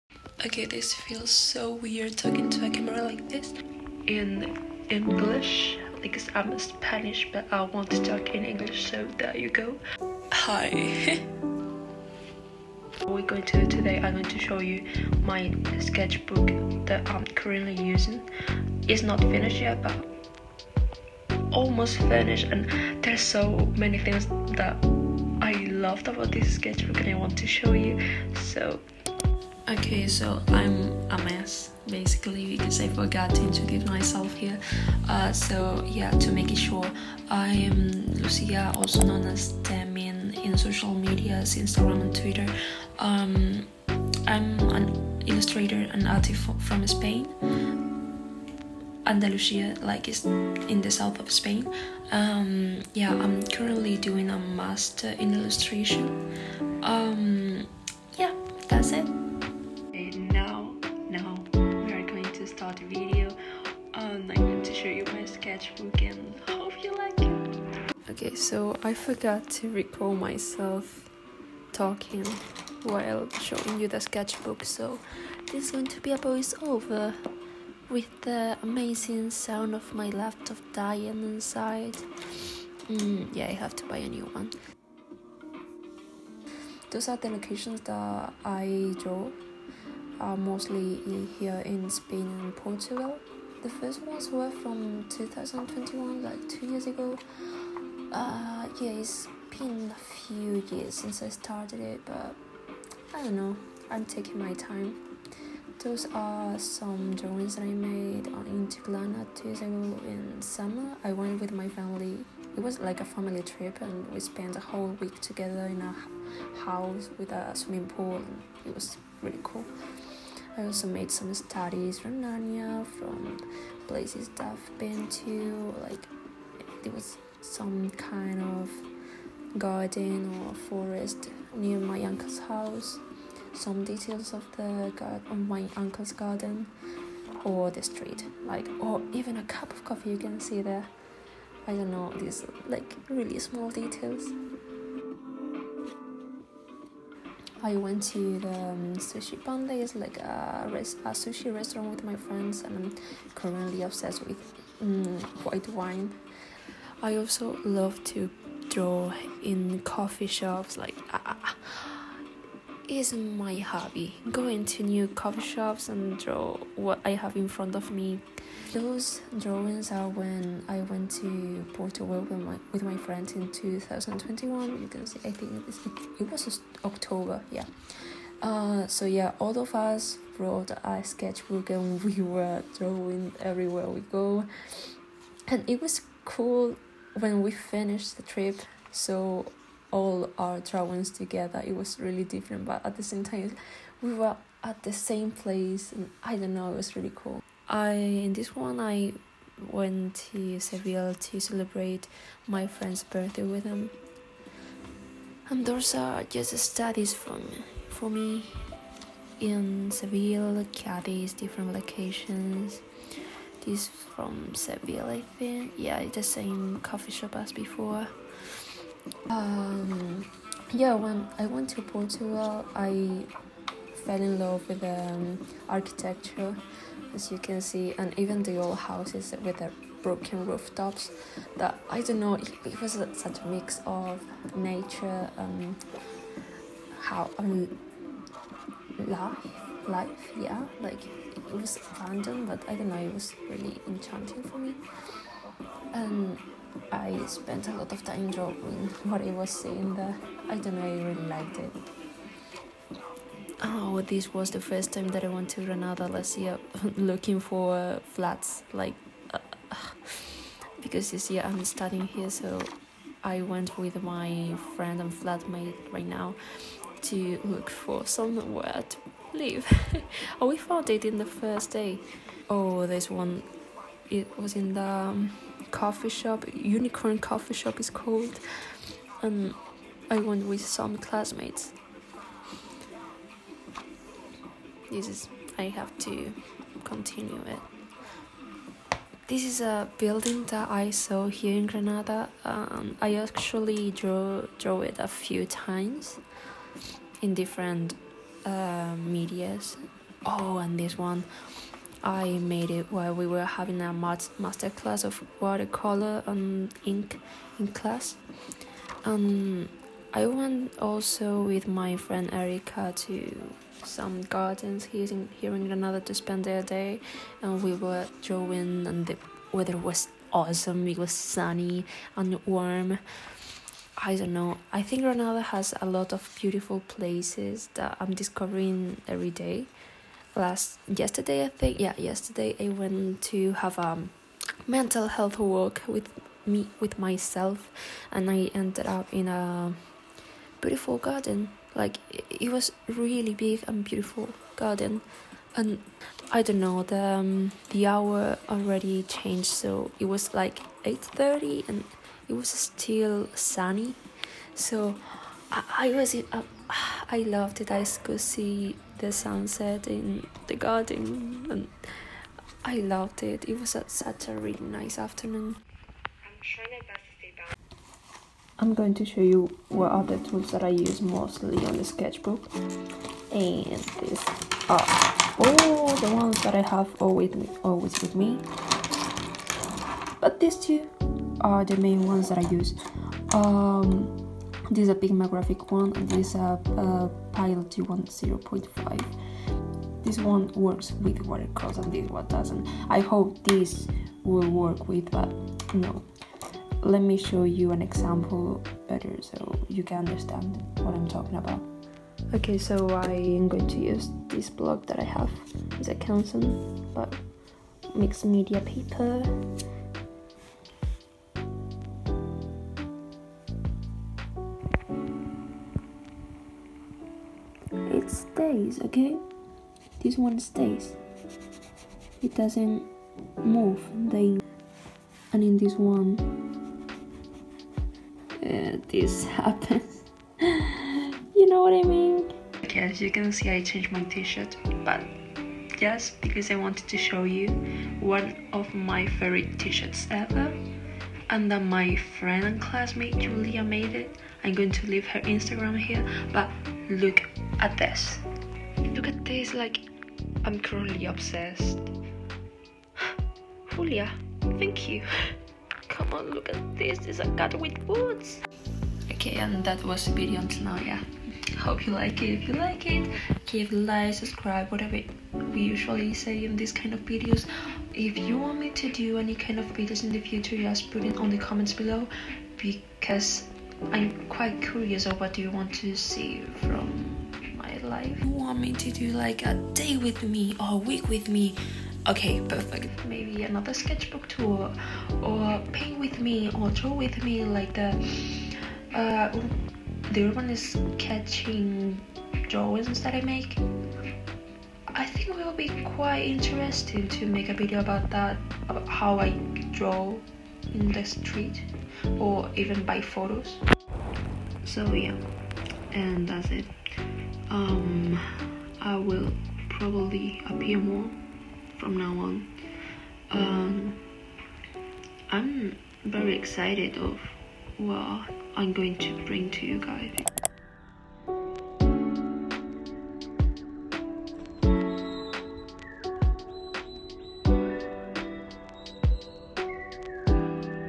sketchbook tour #3 // 2023 - travels and daily life
2023
completely improvised video, this morning i felt inspired to finally talking to the camera and doing a voiceover.
like who are you and what did you do with lucia? I never saw myself with the courage to do this, but this morning I felt inspired. Hope you like it and give it a lot of loveeee

p o r t f o l i o